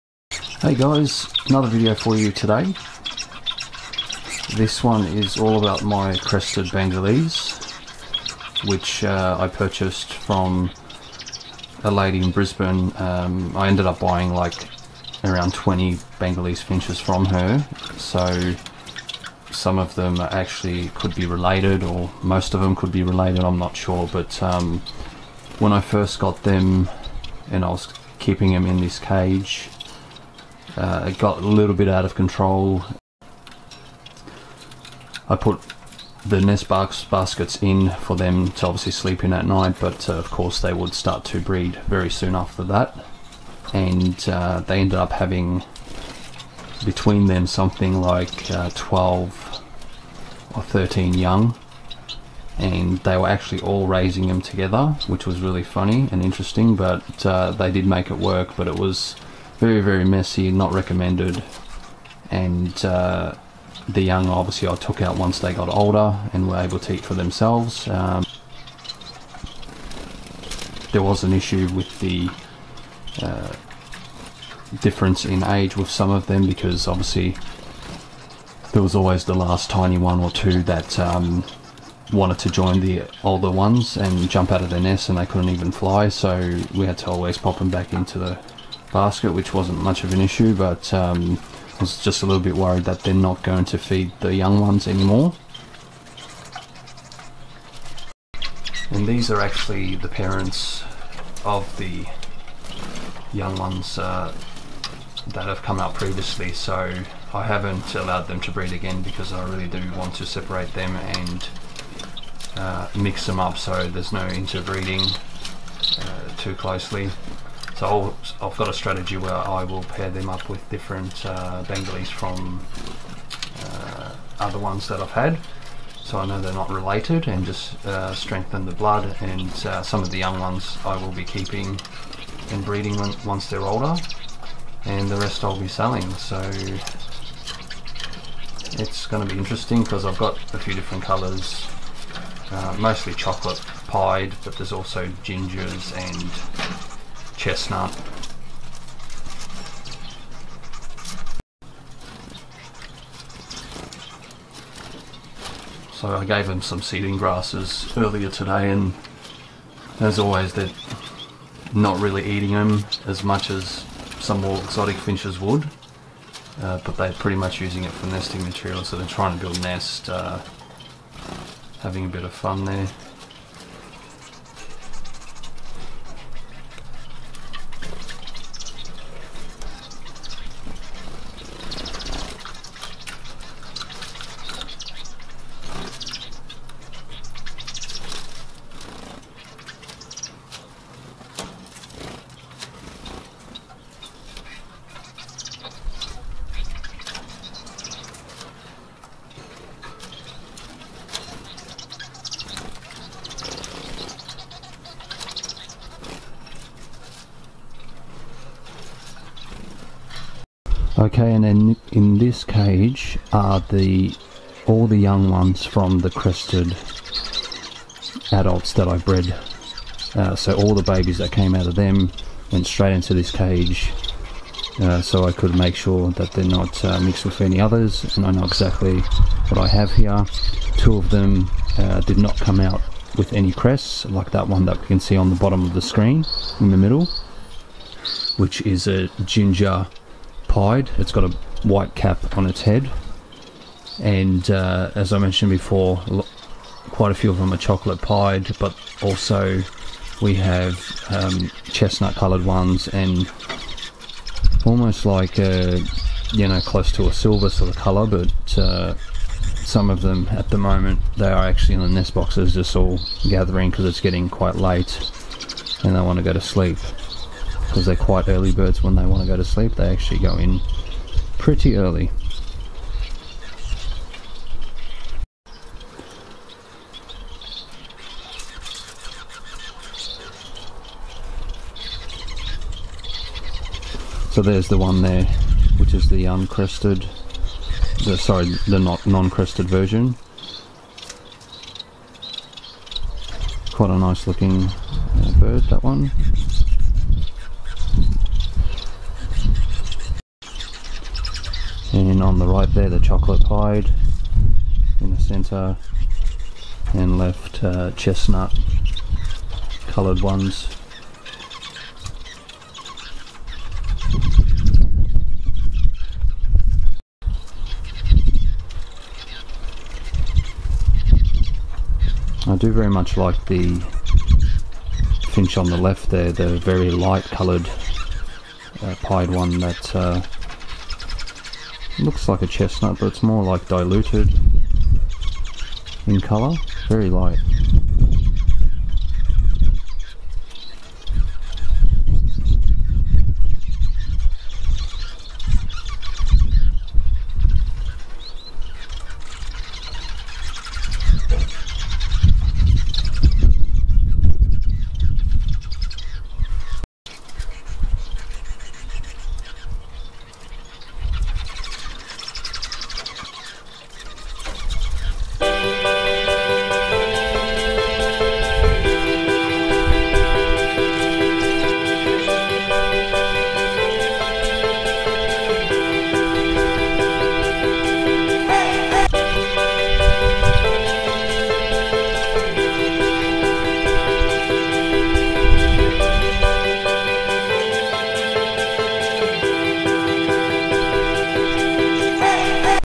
Crested Bengalese Finches
Here are some of my crested varients of the Bengalese Finches.